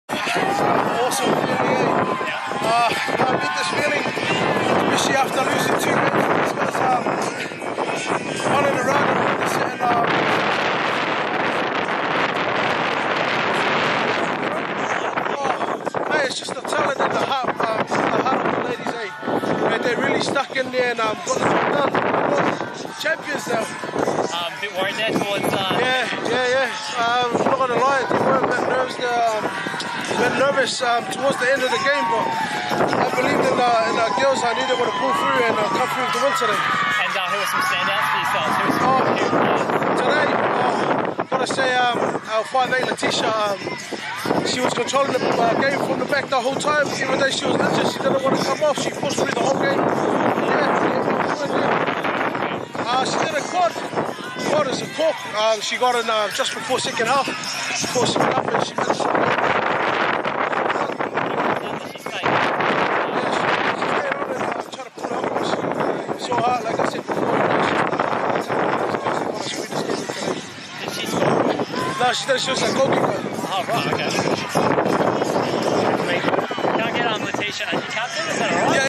Caboolture coach Faafeu Faalogo after his sides win in the 2020 SCRU Women's Grand Final
Contest:
Noosa Dolphins versus Caboolture Snakes
Competition:
2020 Sunshine Coast Rugby Union Women's Grand Final